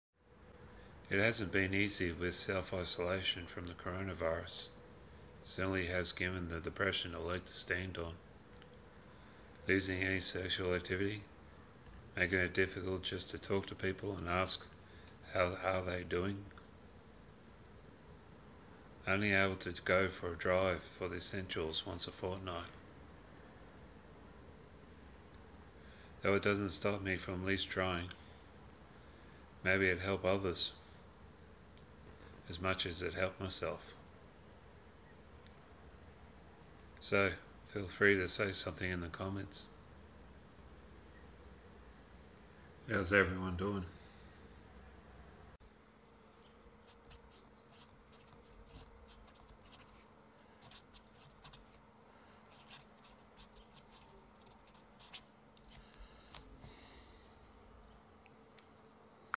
How's Everyone Doing?
Just seeing how all are doing, especially during this self isolation from Caronavirus, may help just to share in the comments below.